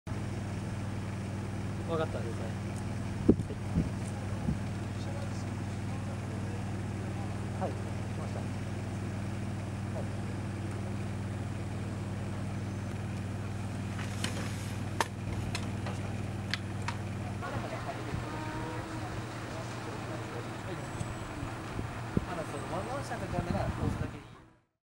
多重事故で高齢男女死亡＝対向車線逆走、７人負傷－福岡
４日午後７時５分ごろ、福岡市早良区百道の交差点で、「複数の車が絡む事故があり、けが人が多数出ている」と１１０番があった。福岡県警早良署によると、乗用車など６台が絡む事故で男女９人が搬送され、うち８０代男性と同乗の７０代女性の死亡が確認された。この男性が運転する車が対向車線を逆走し、多重事故につながったとみられる。【時事通信映像センター】